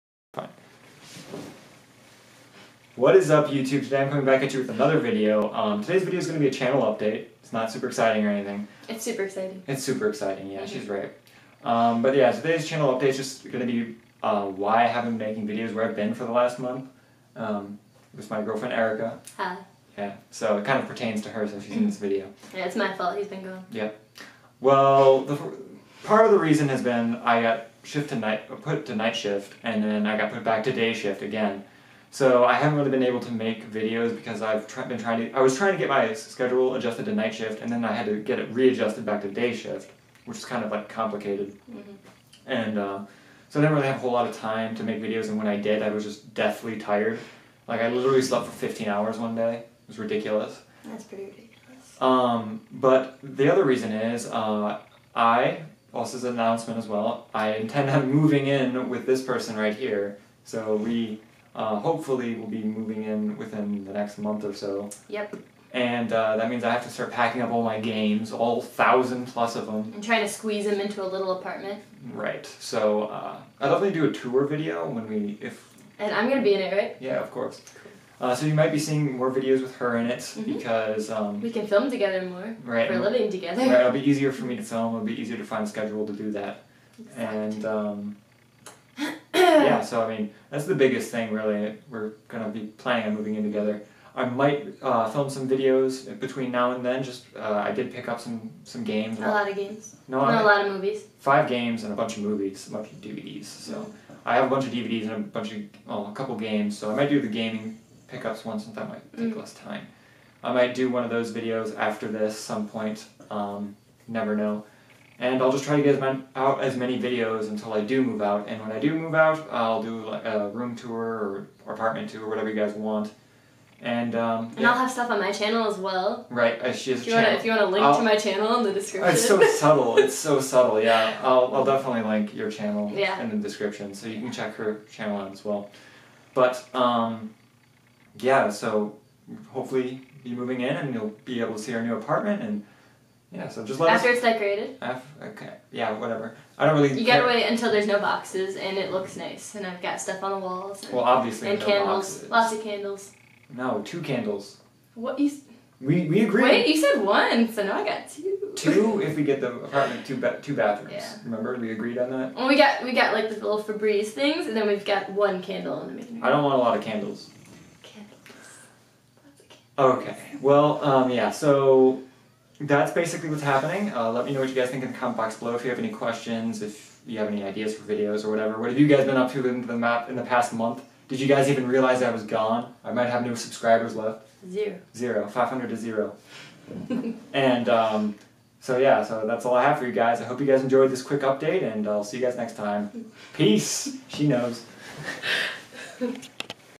Important Channel Update/Where I have been.
Hello again youtube. I just thought I would make a video informing you all as to where I went for the past month and why I stopped making videos. I hope you guys like this news.